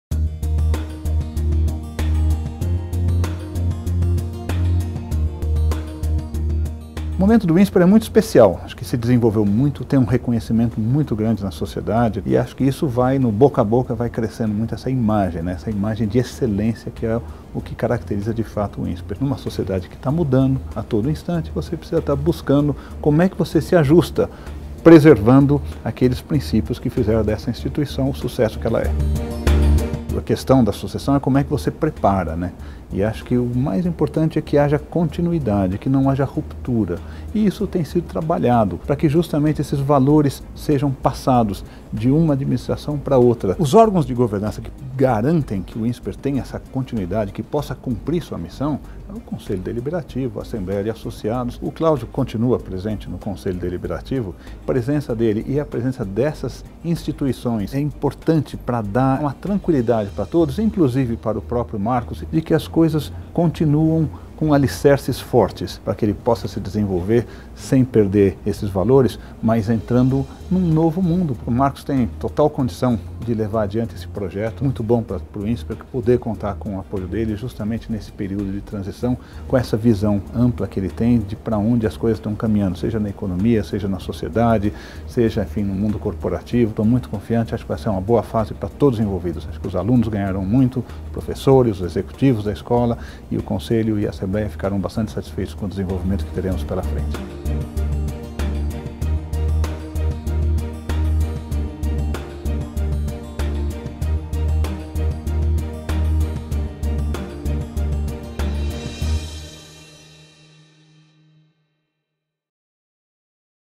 Fábio Barbosa e a sucessão da governança no Insper
Integrante do Conselho Deliberativo do Insper, Fábio Barbosa comenta sobre o processo de sucessão na governança do Insper.